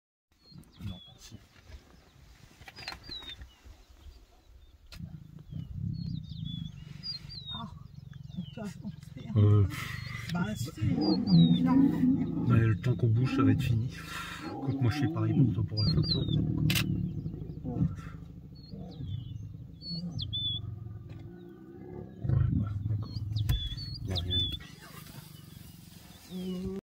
Can You Feel The Love This Morning?
Lions are not quite as romantic as that scene in the Lion King movie